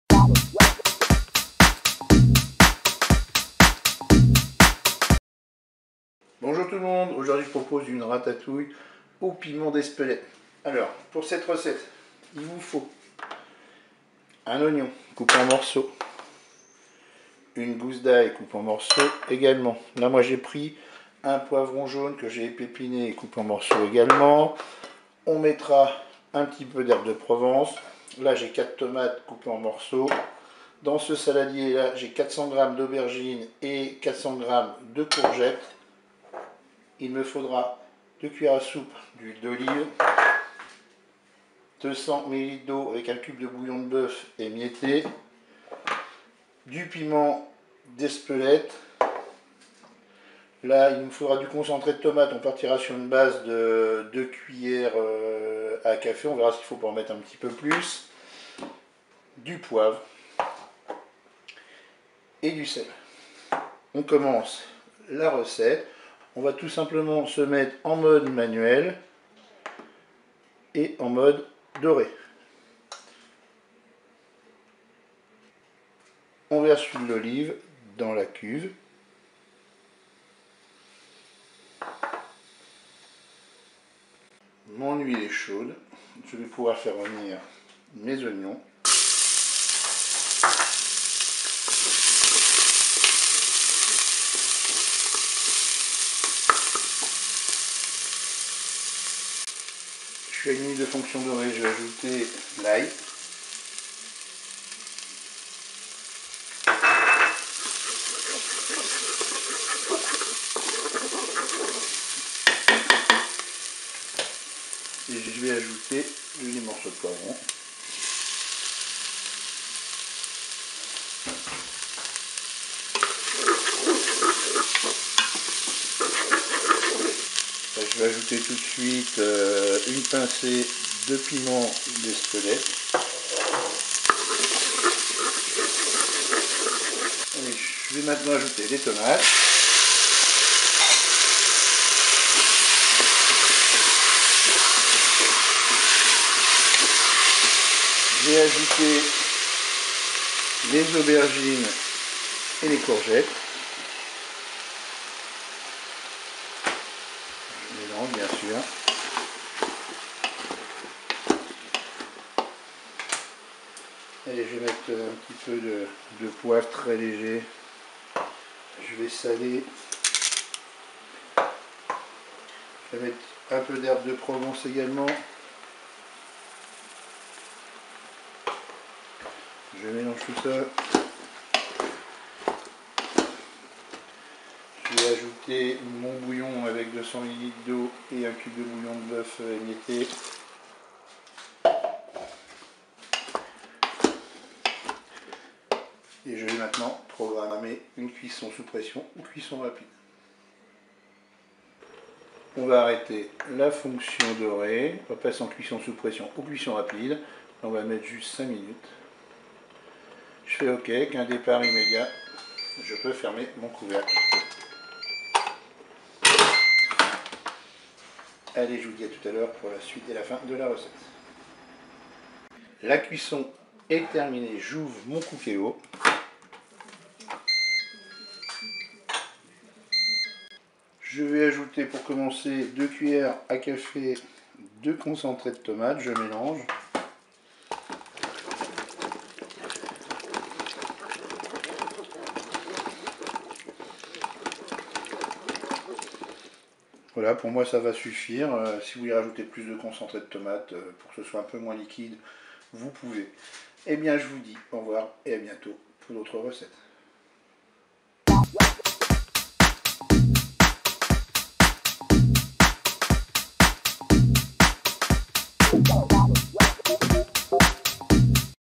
Ratatouille piment d'Espelette recette cookeo
Ratatouille piment Espelette cookeo
Ingrédients pour 4 personnes
1 poivron

4 tomates

1 oignon frais

1 gousse d’ail

400 g d’aubergines

400 g de courgettes

1 cuillère à café d’ail déshydraté ou 2 gousses d’ail

2 cuillères à café d’huile d’olive

1 cuillère à café d’herbes de Provence

Piment d’Espelette

200 ml d’eau

1 cube de bouillon de bœuf

2 cuillères à soupe de concentré de tomates

Sel